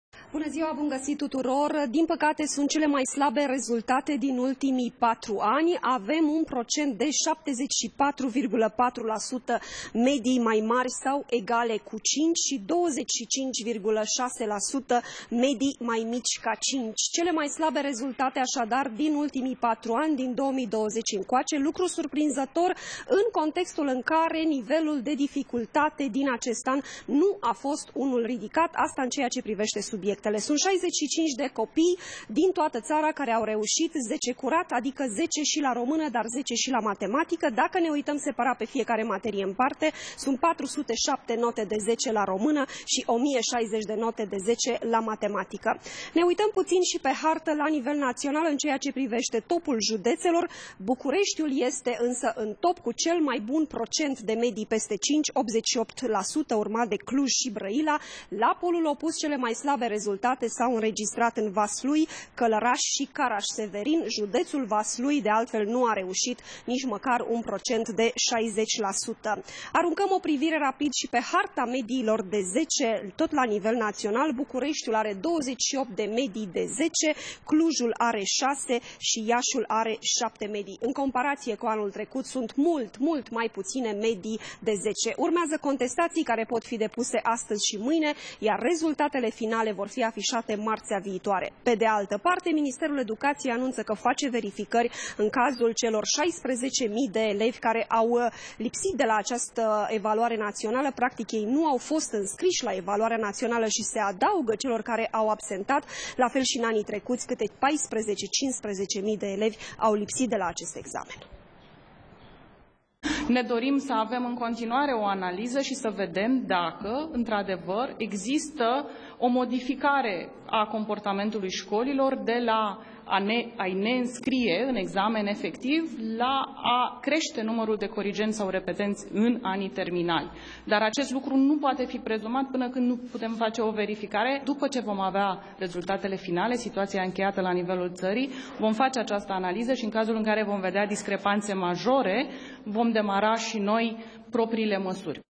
S-au afișat rezultatele la Evaluarea Națională
S-au afișat notele la Evaluarea Națională. Aproape 75 la sută dintre medii sunt peste 5 însă, per ansamblu, rezultatele sunt mai slabe decât anul trecut. De azi se depun și contestațiile, astfel că rezultatele finale vor fi afișate săptămâna viitoare.